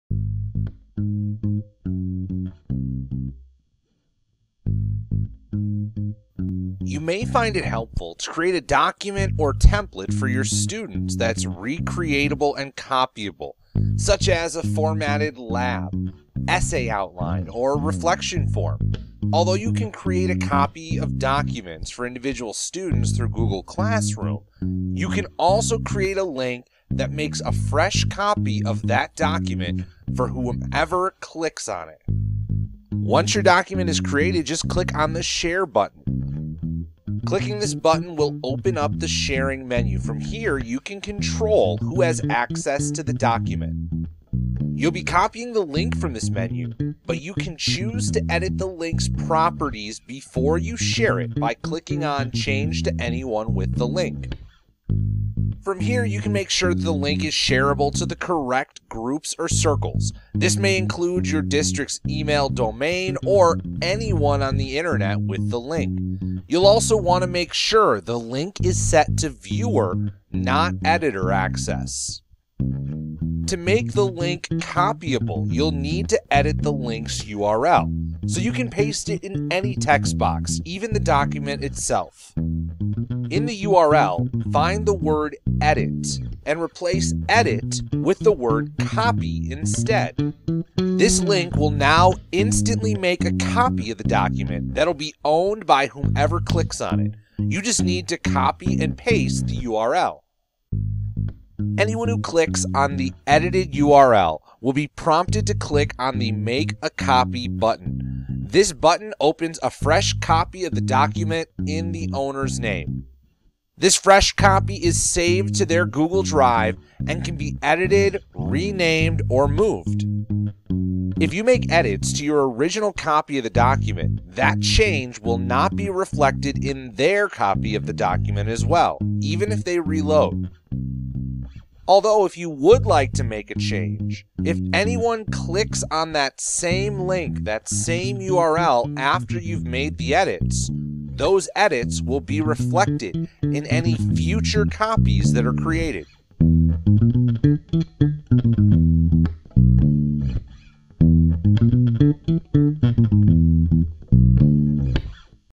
Create a Copy Link in Google Drive
You may find it helpful to create a document or template for your students, such as a formatted lab, essay outline, or reflection form.  Although you can create a copy of documents for individual students through Google Classroom, you can also create a link that makes a fresh copy for whoever clicks on it.

Once your document is created, just click on the share button.

Clicking this button will open up the sharing menu.  From here, you can control who has access to the document.

You’ll be copying the link from this menu, but you can choose to edit the link’s properties before you share it by clicking on “Change to anyone with the link.”

Make sure that the link is shareable to the correct groups.  This may include your district’s email domain or anyone with the link.  You’ll also want to make sure the link is set to Viewer, not Editor, access.

Once your settings match your needs, click on the Copy Link button and click “Done” to close out the sharing menu.

You’ll need to edit the link’s URL, so you can paste it in any text box, even your document.

In the URL, find the word “Edit” and replace edit with the word copy instead.

This link will now makes an instant copy of the document that will be owned by whomever clicks on it.  You just need to copy and paste it.

Anyone who clicks on this link will be prompted to click on the “Make a Copy” button.  This opens a fresh copy of the document.

This fresh copy is saved to their Google drive and can be edited and renamed.

If you make edits to your original copy of the document, that change will not be reflected in their copy of the document.

Although, if someone clicks the same link after you’ve made edits, those edits will be reflected in the fresh copy that is created.